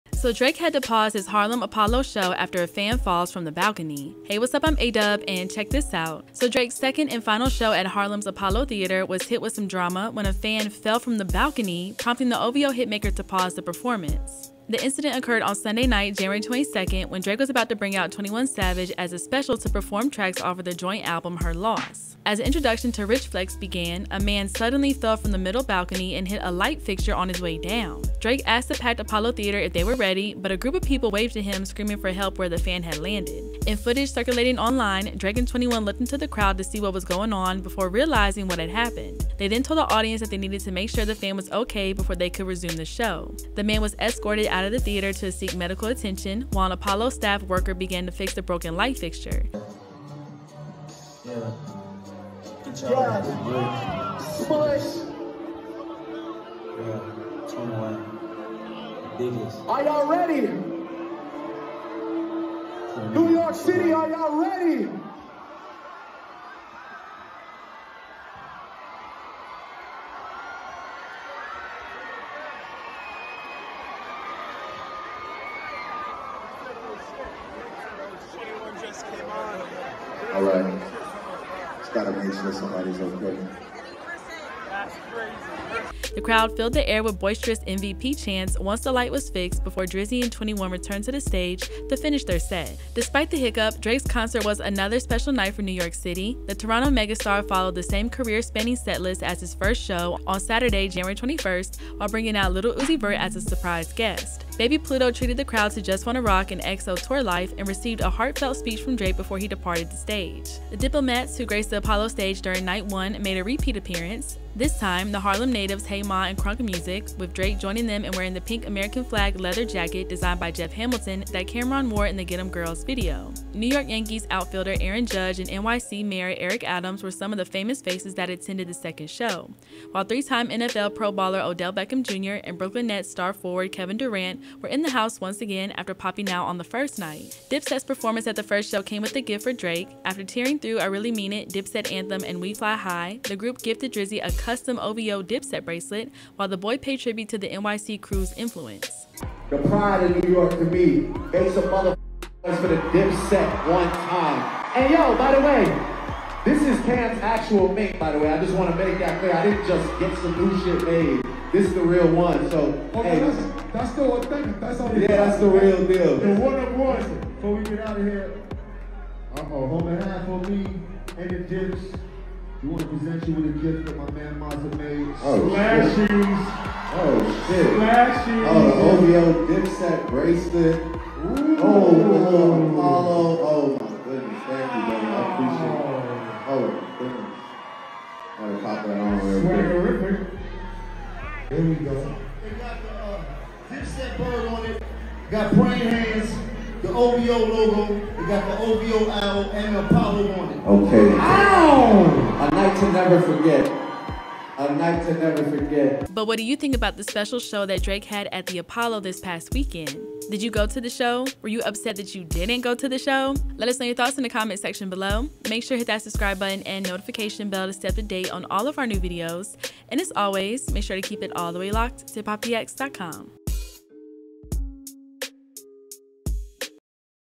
Drake Pauses Performance After Someone Fell From Balcony At The Apollo in NYC
Script Written By: Mark Elibert
Video Edit: Mahib Balde